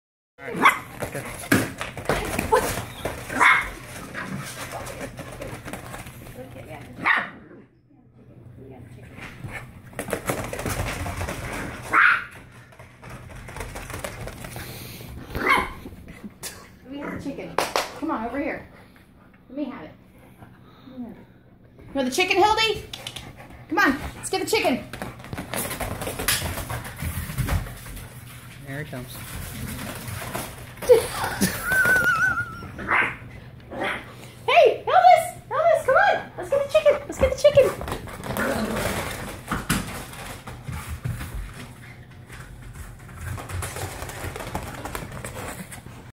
Dogs getting the Chicken
Our 3 dogs, including little Schtuka, 8 weeks old fetching the plastic chicken.